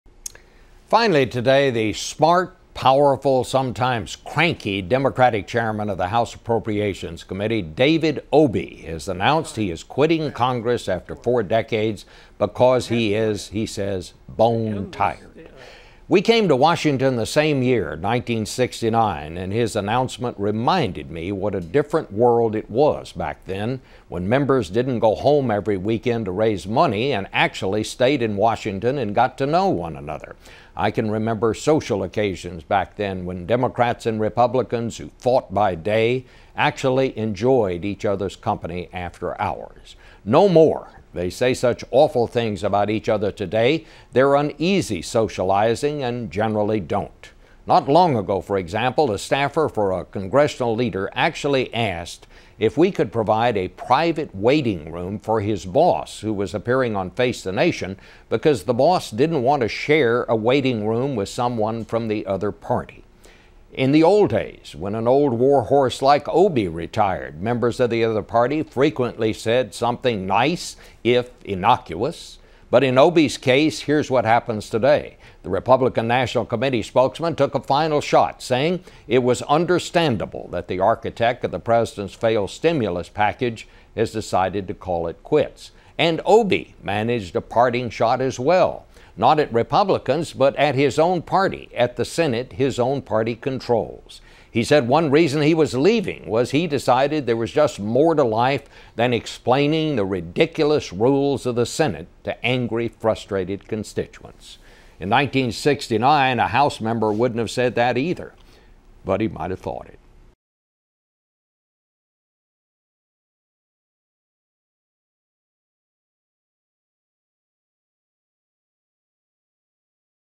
David Obey Quits Congress
Bob Schieffer reflects on the career of now former Democratic Chairman of the House Appropriations Committee David Obey after serving forty years in office.